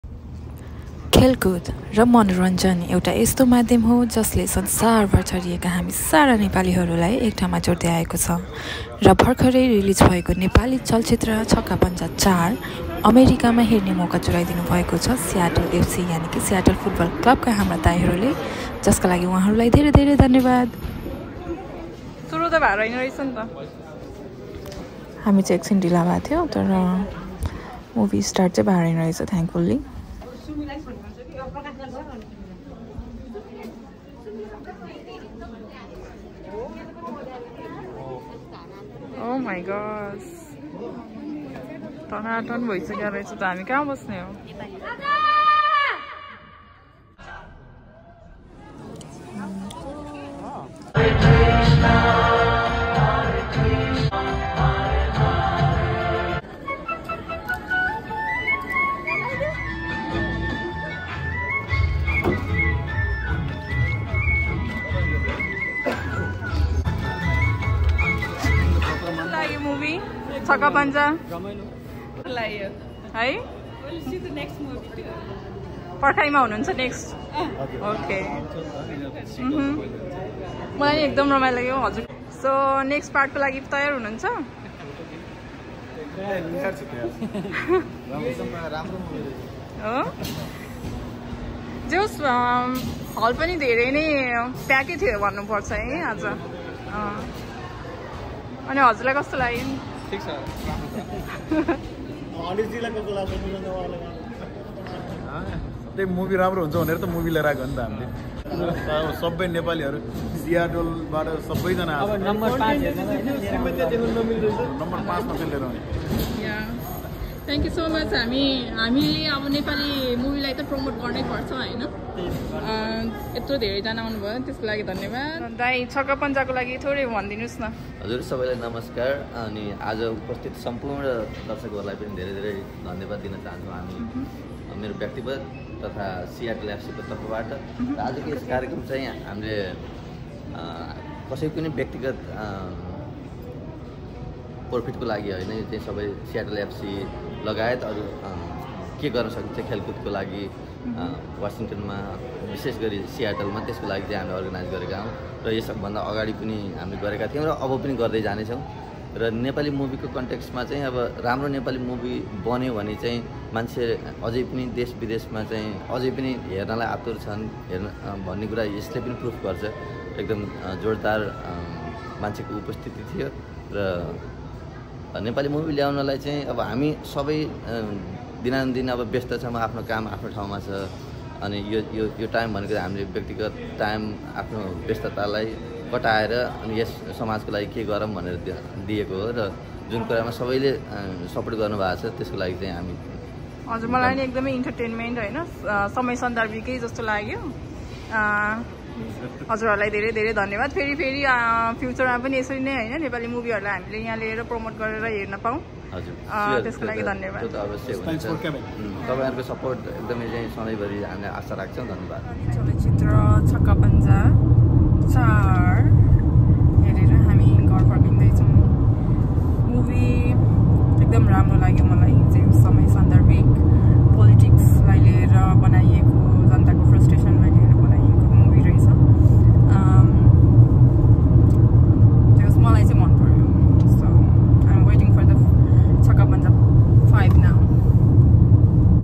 Chhakka Panja 4 in America | Audience Reaction | Movie Review | Full Entertainment
Promote Nepali Movie ❤️